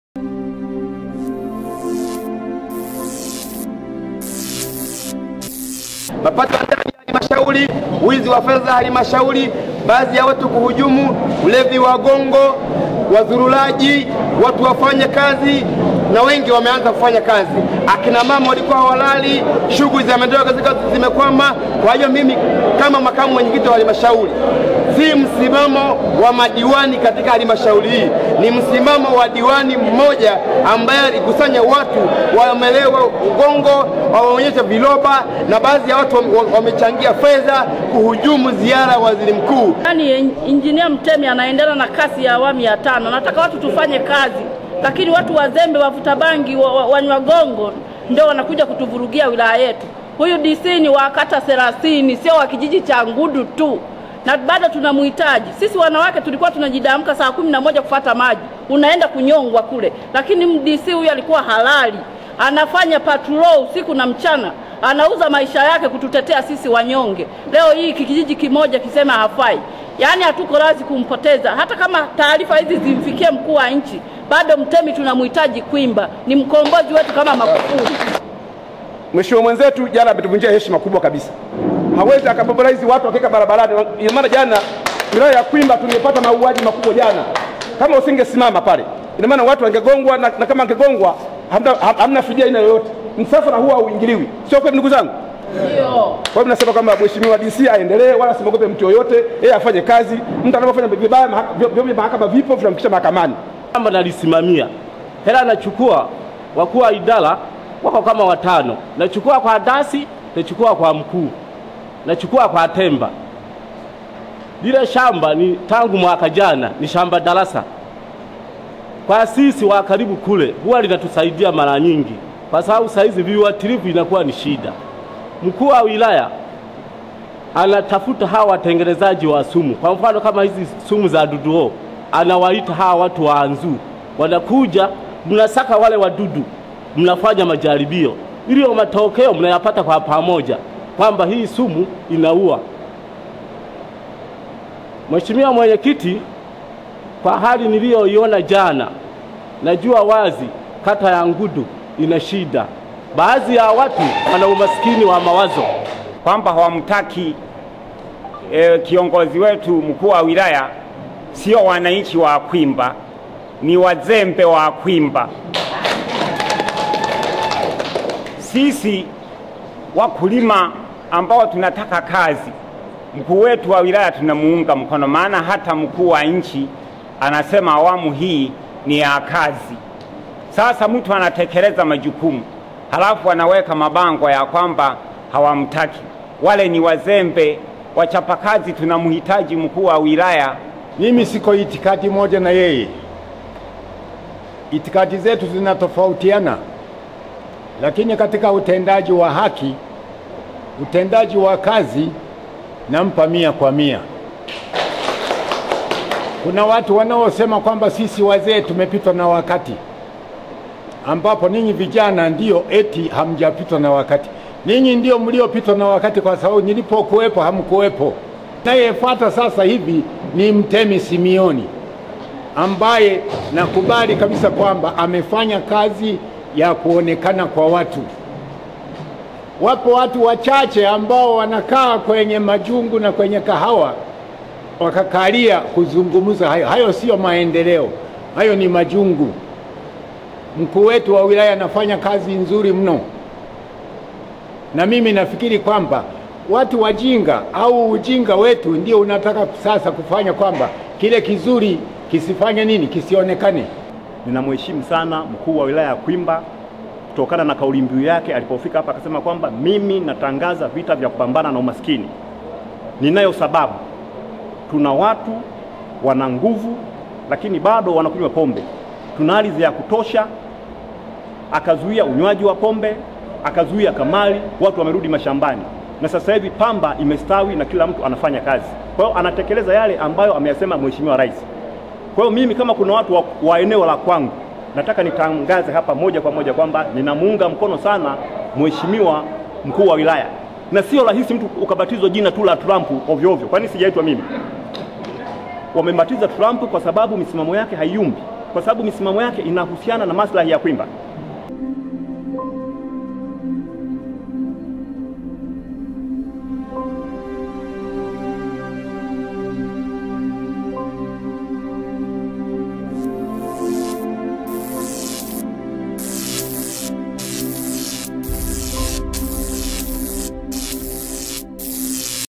IMEGUNDULIKA KUWA WANAOMPINGA MKUU WA WILAYA YA KWIMBA NI WAZEMBE NA WAZURURAJI
Madiwani wa halmashauri ya wilaya kwimba mkoani Mwanza wamepinga madai kuwa mkuu wa wilaya hiyo Mhandisi Mtemi Simioni amekuwa akiwaamrisha Polisi kuwakamata baadhi ya wananchi wa kata ya Ngudu kwa kisingizio cha uzururaji na kisha kuwalimisha kwenye shamba lake.

Ni Siku mbili baada ya baadhi ya wananchi wa kata ya Ngudu wilayani kwimba mkoani Mwanza kuzuia msafara wa waziri mkuu Kasimu Majaliwa wakitaka kuondolewa kwa mkuu wa wilaya hiyo mhandisi MTEMI SEMEON kwa madai kuwaamrisha polisi kuwakamata kwa kisingizio cha uzururaji na kisha kuwalimisha kwenye shamba lake.

Kufuatia malalamiko hayo ya baadhi ya  wananchi ,Madiwani wa halmashauri ya Kwimba mkoani Mwanza ambao wamekutana na vyombo vya habari  wamesema kuwa madai hayo si ya kweli na kwamba wananchi wa ngudu waliosimamisha msafara wa waziri mkuu KASIMU MAJALIWA wanatumiwa na kikundi cha watu kisichotaka watu kufanya kazi pamoja na wale waliokuwa wakijipatia fedha za umma kwa njia za udanganyifu kabla ya mianya hiyo kuzibwa na mkuu huyo wa wilaya.

Hawa ni baadhi ya Madiwani wa halmashauri hiyo ya Kwimba wakiongozwa makamu mwenyekiti wa halmashauri hiyo Shija Malando ambao kwa pamoja wakatoa msimamo wao kuhusu kitendo cha diwani wa kata ya Gudu Juston Malifedha.

Madiwani hao ambao wamejitokeza mbele ya vyombo vya habari wakaeleza kiini cha sintofahamu iliyojitokeza katika ziara ya ya mhe.kassim Majaliwa Kassim.